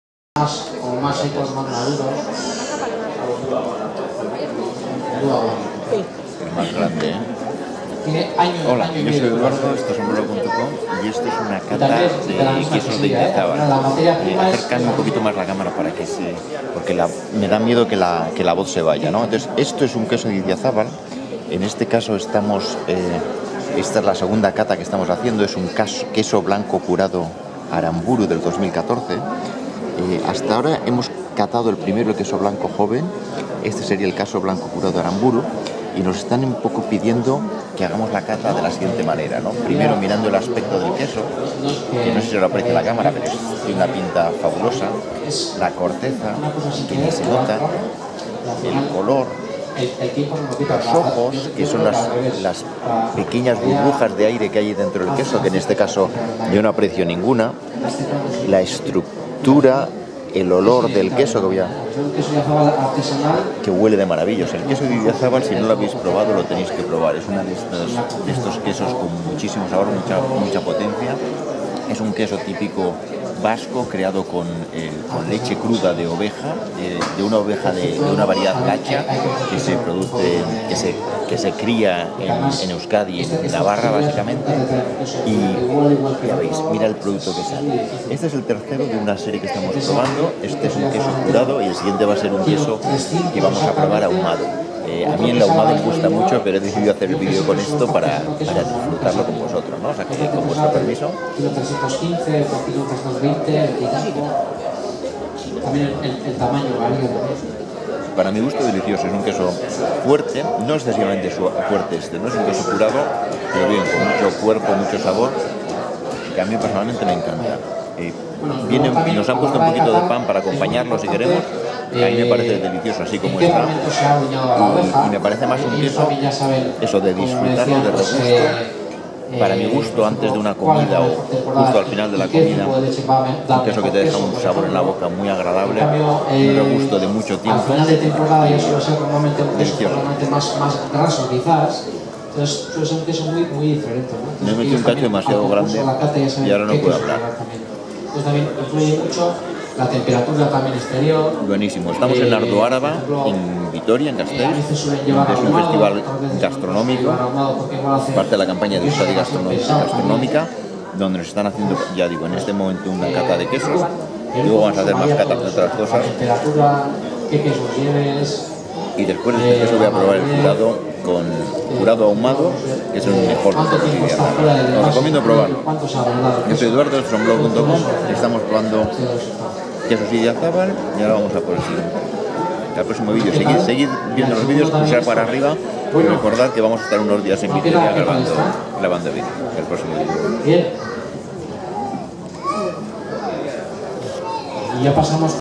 Cata de queso Idiazabal en Ardo Araba
- Nos han invitado a catar quesos de Idiazabal en las celebraciones de Ardo Araba en Vitoria. Una maravilla de queso, con un sabor fuerte pero delicioso. Mi favorito el clásico, el Idiazabal ahumado.